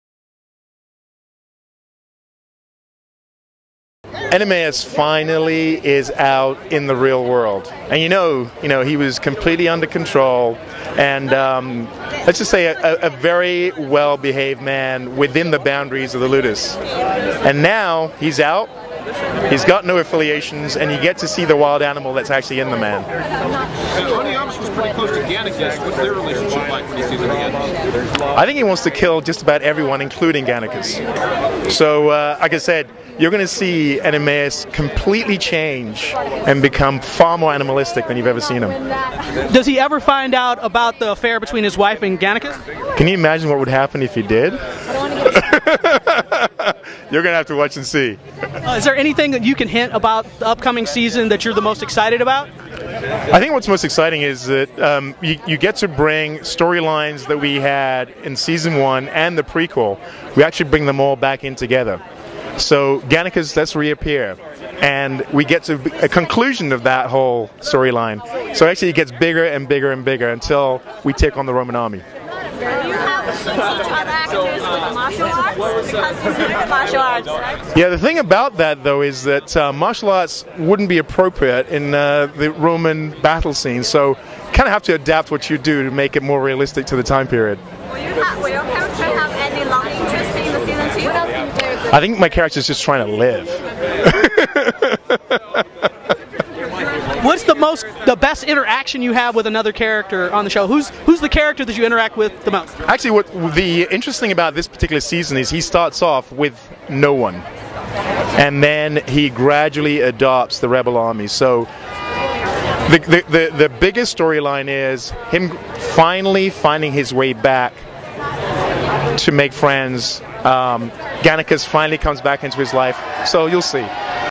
Spartacus Red Carpet: Peter Mensah (Oenomaus)
Interview with Peter Mensah (Oenomaus) from Spartacus